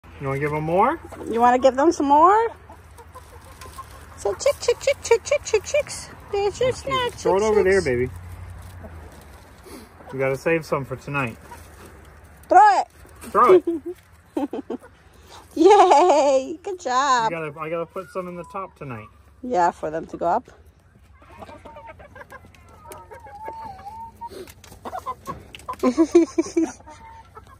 Catalina loving on her chickens 😃❤️🐓
Catalina loves feeding her chickens and giving them snacks.